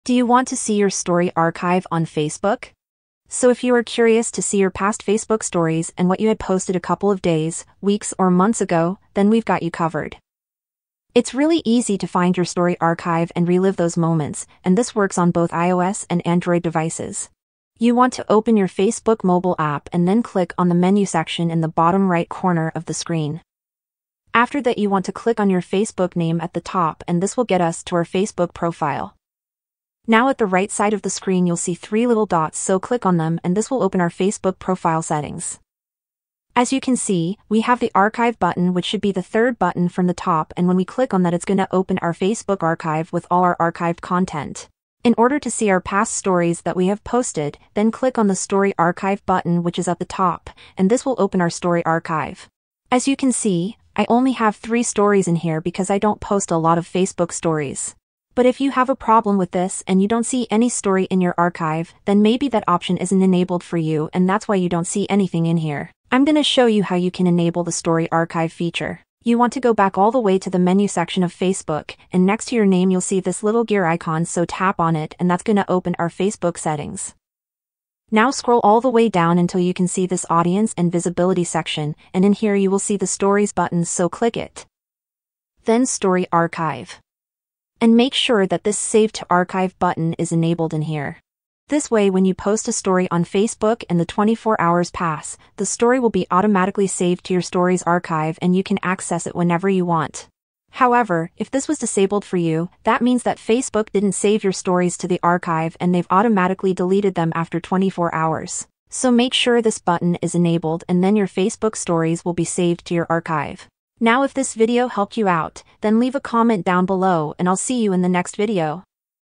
How To See Story Archive In Facebook
Are you struggling to find your past stories on Facebook? Well, worry no more! In this tutorial, we'll show you exactly how to access your Story Archive on Facebook and relive those precious moments with just a few simple steps!